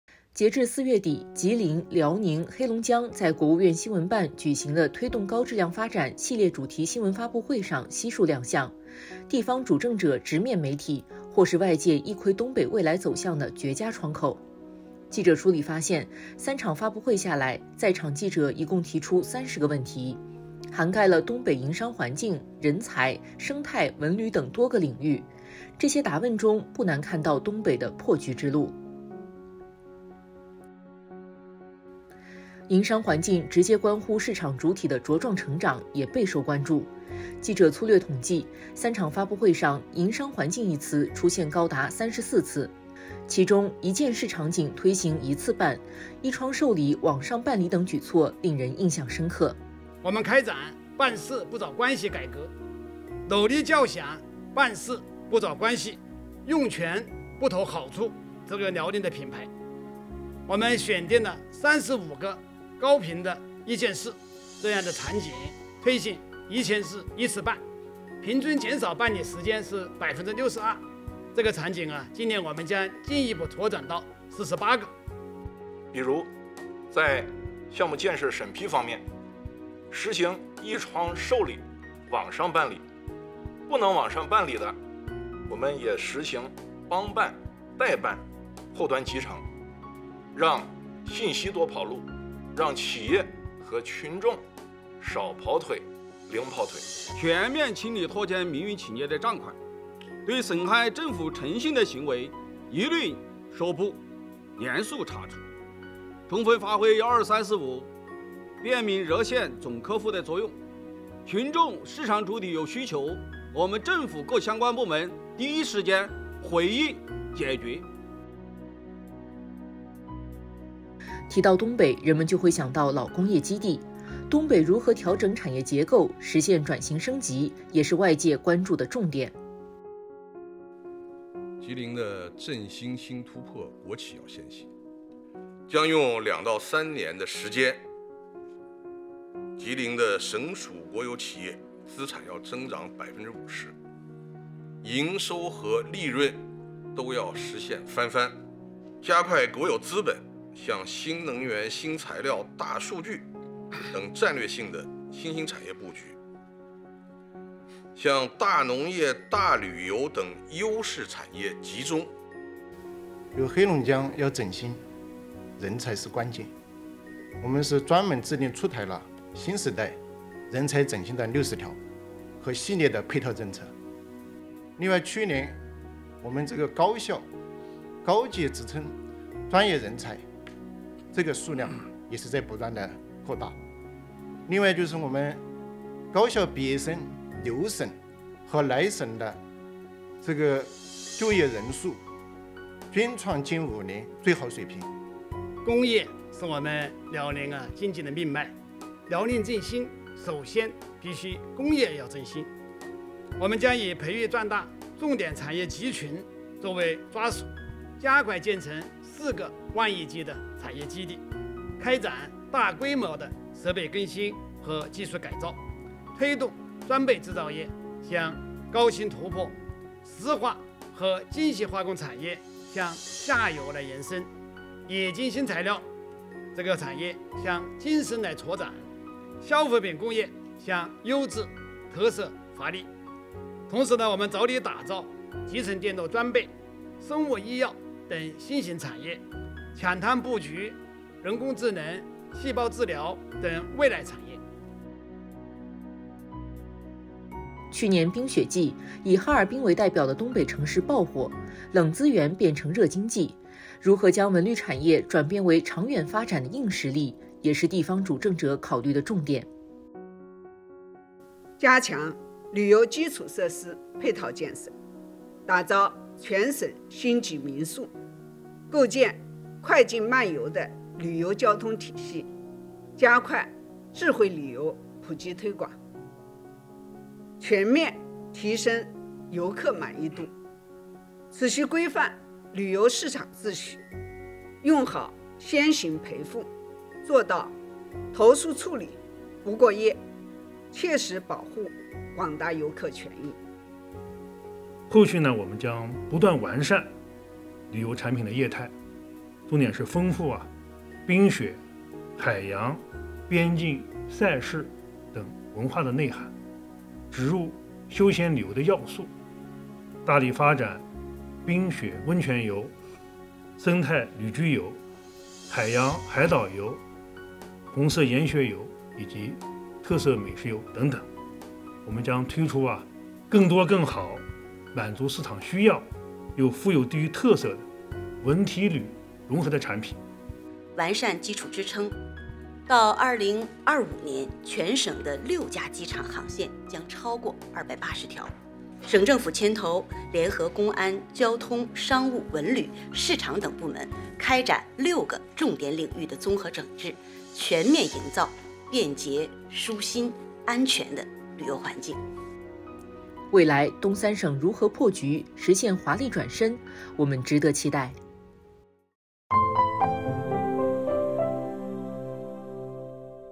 东北“新局”如何破？三场高规格发布会释放什么信号？
截至4月底，吉林、辽宁、黑龙江在国务院新闻办举行的“推动高质量发展”系列主题新闻发布会上悉数亮相。地方主政者直面媒体，或是外界一窥东北未来走向的绝佳窗口。

记者梳理发现，三场发布会下来，在场记者一共提出30个问题，涵盖了东北营商环境、人才、生态、文旅等多个领域。这些答问中，不难看到东北的“破局之路”。（刘超）